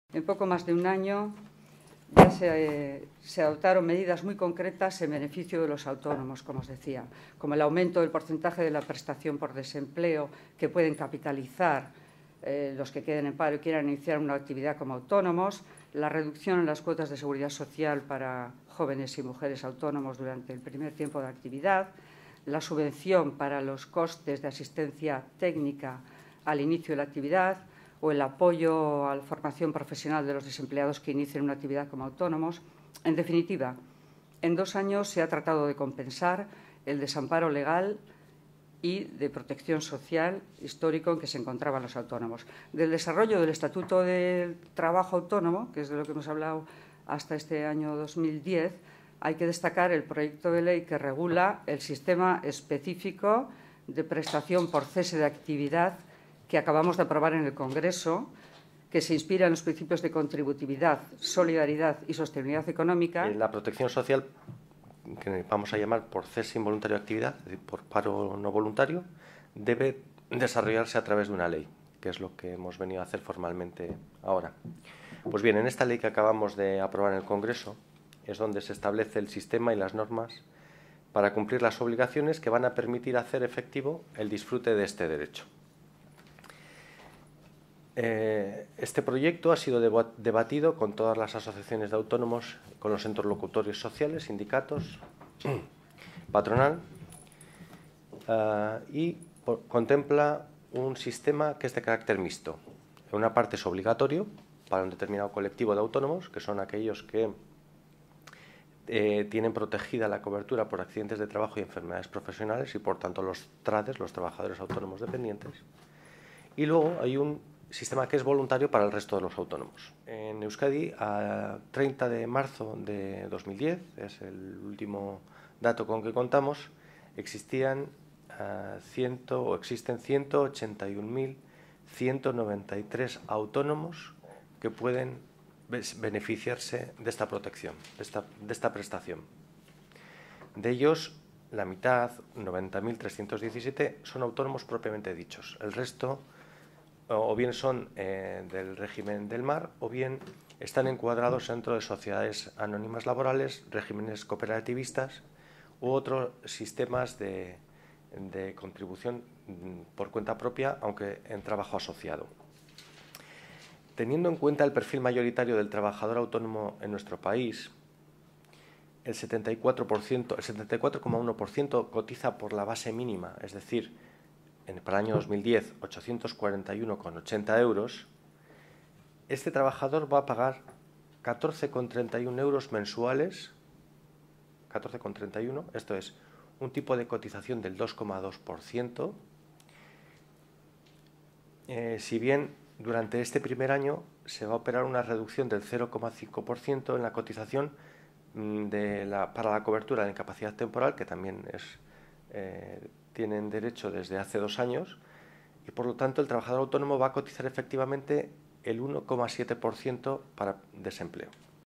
Prestación Autónomos
Rueda de prensa de Carmen Achútegui y Oscar Seco para hablar de la nueva ley que regula la prestación de los autónomos